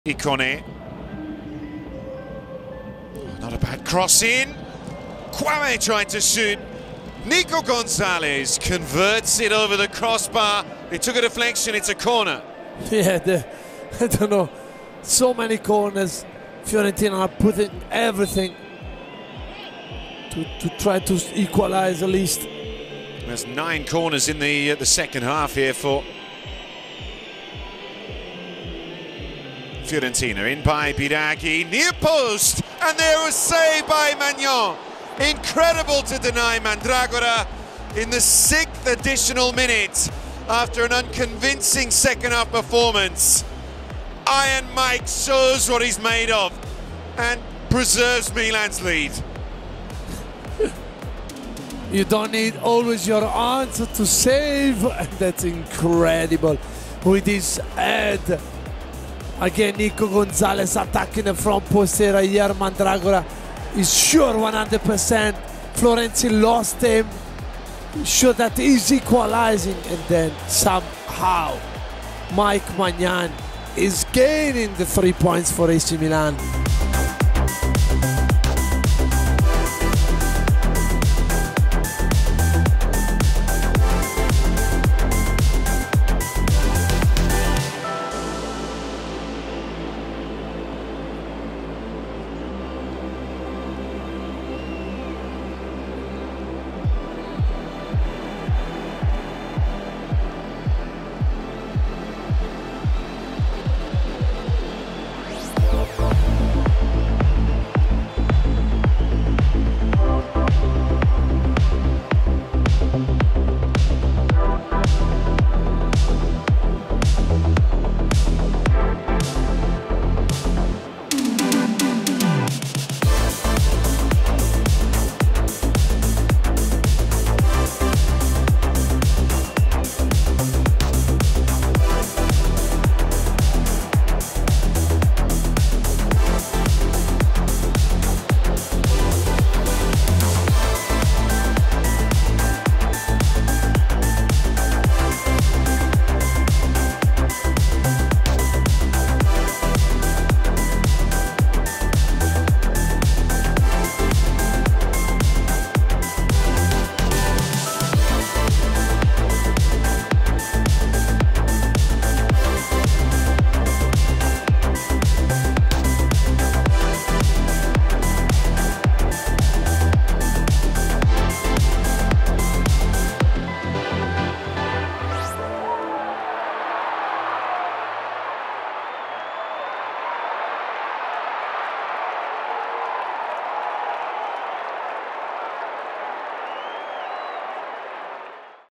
Maignan’s unorthodox save | Every Angle | Milan-Fiorentina | Serie A 2023/24
Milan were hanging onto their narrow lead against Fiorentina and as La Viola was pushing to find the equaliser in the latter stages of the game, the French keeper pulled off a crucial save from Mandragora to keep the score level | Serie A 2023/24